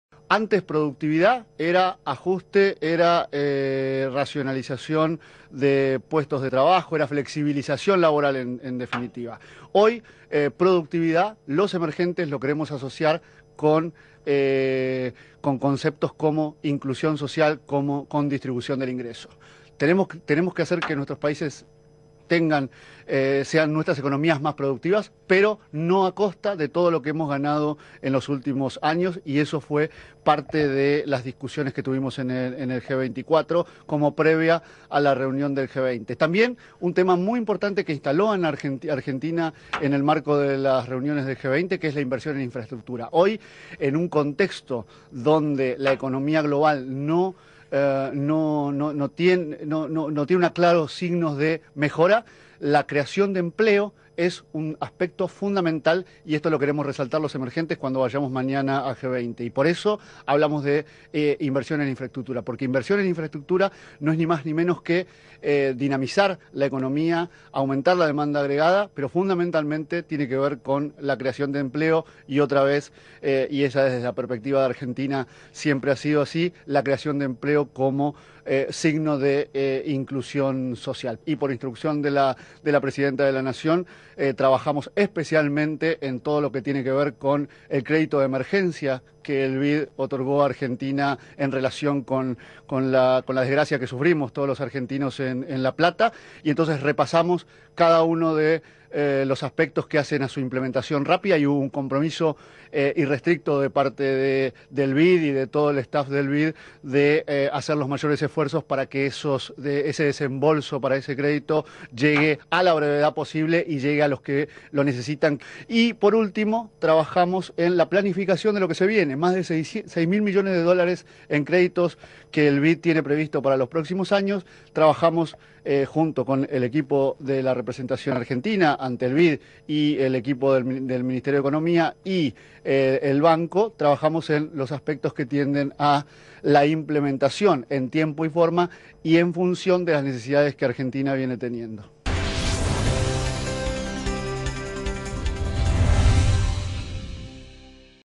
"Queremos que el empleo y la inclusión social sean prioridad para el FMI y el Banco Mundial"
El ministro de Economía, Hernán Lorenzino, se pronunció hoy a favor de que el Fondo Monetario Internacional (FMI) y el Banco Mundial (BM), incluyan como temas prioritarios en sus agendas al "empleo y la inclusión social".

El ministro, en declaraciones realizadas en Washington, donde participa de las reuniones de primavera boreal del FMI y Banco Mundial, destacó hoy la decisión del Grupo de los 24 (G24) que, a instancias del país, destacó la necesidad de de la "inversión en  infraestructura" como una forma de "aumentar la demanda agregada" e impulsar "la creación de empleo como signo de inclusión social".

"Hoy en un contexto donde la economía global no tiene claros signos de mejora, la creación de empleo es un aspecto fundamental y esto queremos resaltar los (países) emergentes" en la reunión del Grupo de los 20 en la que participará mañana el ministro, en el marco de la cumbre que reúne a 188 países.

En la reunión del G-24, fue discutido otro tema que Argentina viene planteando en los últimos años, como es "la situación general de la economía global y en este sentido la ratificación una vez más de que los países emergentes son el motor de la economía global" pero además "aspectos importantes como la productividad de nuestros países".

"Pero una productividad con una perspectiva de países emergentes.  Antes productividad era ajuste, era racionalización de puestos de trabajo, flexibilización laboral; hoy productividad, los emergentes lo queremos asociar con conceptos como inclusión social, distribución del ingreso", aseguró Lorenzino.

Por la tarde, el ministro de Economía mantuvo un encuentro con el presidente del Banco Interamericano de Desarrollo (BID), Luis Alberto Moreno, junto a quien repasó "el estado de la cartera actual de Argentina, de más de 10 mil millones de dólares en créditos".

Ambos funcionarios, trabajaron "especialmente en todo lo que tiene que ver con el crédito de emergencia que el BID otorgó a Argentina" para hacer frente a las consecuencias de la inundación que afectó a la ciudad de La Plata el último 2 de abril.

Por su parte, la entidad interamericana dio su "compromiso irrestricto de hacer los mayores esfuerzos para que ese desembolso para ese crédito llegue a la brevedad posible y a los que necesitan" tras la inundación que afectó a la capital de la provincia de Buenos Aires, comentó el funcionario argentino a la prensa.

En el encuentro, avanzaron además en la "planificación de los más de 6.000 millones de dólares en créditos que el BID tiene previstos para los próximos años" así como en "los aspectos que tienden a la implementación en tiempo y forma y en función de las necesidades que la Argentina viene teniendo".

En la agenda con Moreno, fueron abordados también la situación de "otros programas que están siendo incluidos en la cartera y otros que pensamos priorizar" como el destinado para la reconstrucción ambiental del río Reconquista.

La sede del BID fue el escenario donde Lorenzino inició sus actividades una vez llegado a los Estados Unidos, con una reunión junto al titular de la institución y a ministros de América Latina para "analizar los problemas de la región con una mirada desde la propia región y pensando en soluciones adaptadas a las realidades de cada uno de nuestros países".

Por otro lado, Lorenzino hizo referencia a los reportes del Fondo Monetario sobre países de la región "y en particular Argentina", que hablaron respecto "a recomendaciones, recetas y tantas veces equivocaron el camino".

"Cuando vemos los reportes de hoy nos acordamos de las recetas de ajuste ni bien empezábamos a salir de la crisis", dijo.

"Sin embargo la receta del Fondo en ese momento fue: hay que enfriar las economías y hoy estamos viendo como los mismos economistas reconocen que tal vez enfriaron demasiado y hay que volver sobre las políticas de estímulo de la economía".

También dijo que cuando "hablamos de calificadoras de riesgo, que eran en los reportes del Fondo actores ineludibles del mercado, hoy vemos cómo países como Estados Unidos o Europa están revisando sus legislaciones justamente en contra de estas políticas del Fondo".

Con todo esto, "vemos en definitiva, una vez más, cómo el Fondo vuelve a dar recetas que luego se demuestran en la práctica que no cumplen con el objetivo que tenían planteados y ahora vemos cómo se revisan esas mismas recetas por el propio staff del FMI", concluyó el ministro.